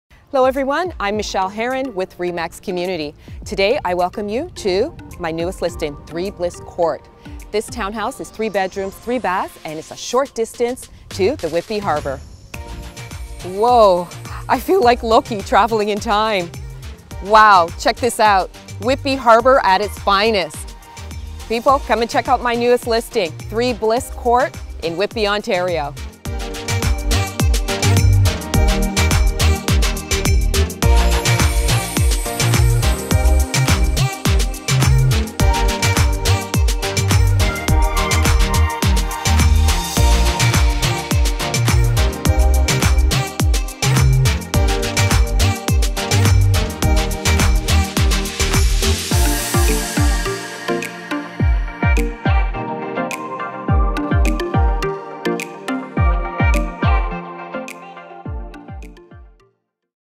3 Bliss Crt, Whitby | TownHouse For Sale in Whitby Ontario - NOW SOLD!
Lovingly kept 2 storey 3 bedroom, 3 bath townhouse with single garage in Whitby, Ontario. Family friendly neighbourhood on a cul-de-sac in the community of Port Whitby. Walking distance to trails and the Whitby harbour. Lots of upgrades to see from 9' ceilings on main level, hardwood flooring throughout, newer appliances, truly a must see!